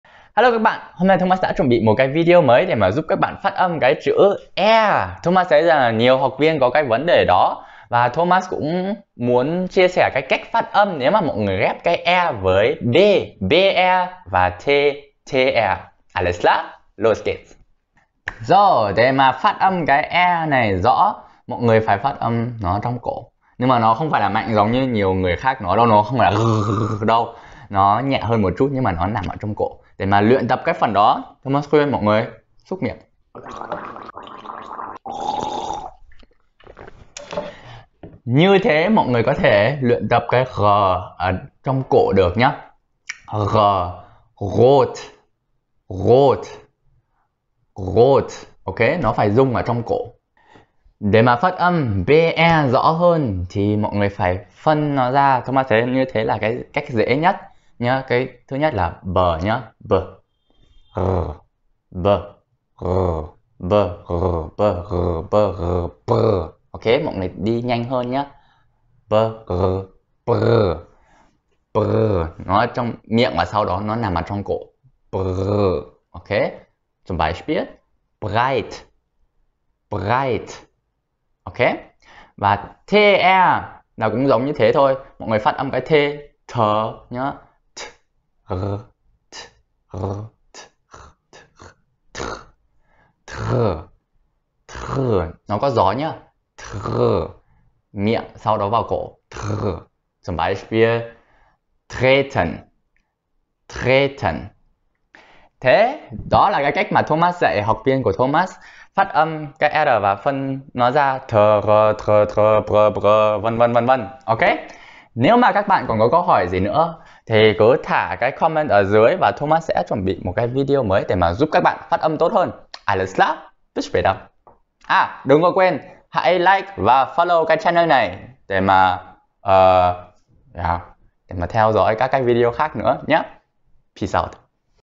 Aussprache: R - Br - Tr | cách luyện phát âm chữ R trông tiếng Đức
Thomas nhận thấy rất nhiều học viên gặp phải vấn đề khi phát âm chữ "R", đặc biệt là với những từ có "br" hoặc "tr". Vì vậy, Thomas đã chuẩn bị một video, qua đó sẽ hướng dẫn các bạn cách luyện tập phát âm hiệu quả. 😉🇩🇪

✨ luyện phần noi​ online và được tặng $10 khi đắng ký một buổi